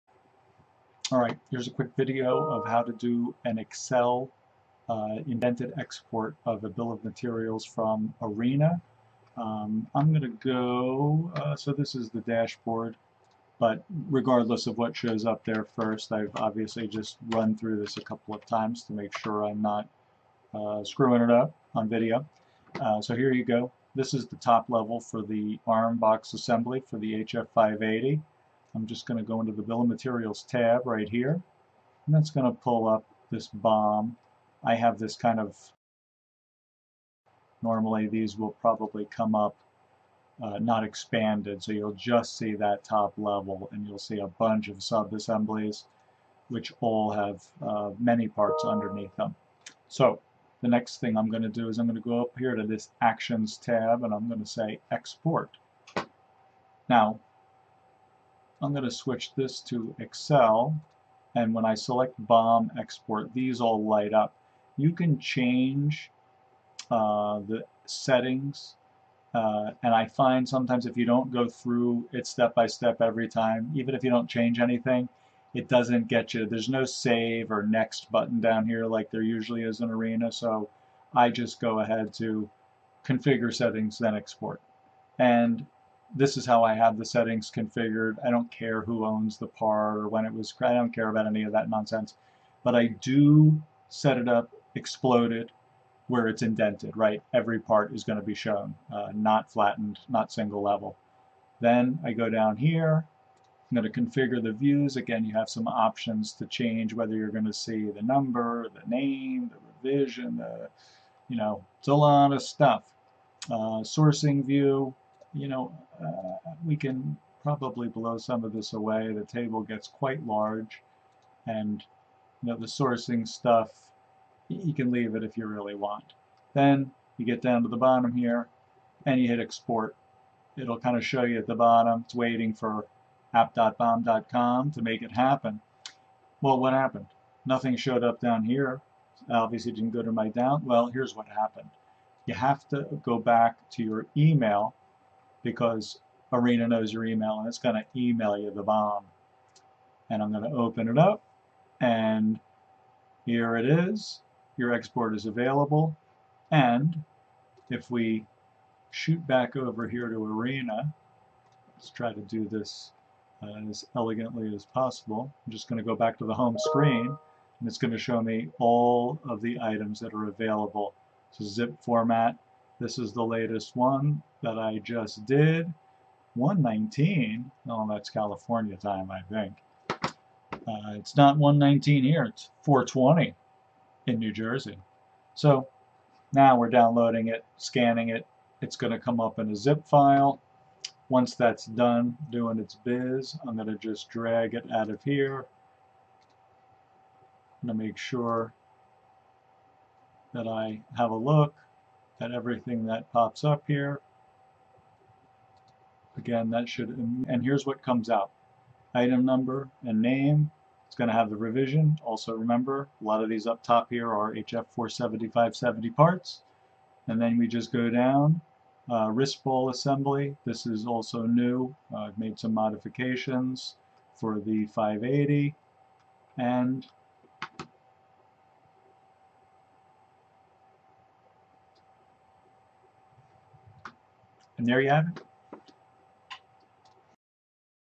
Arena BOM
A not so quick explanation of how to export your BOM from Arena in Excel format.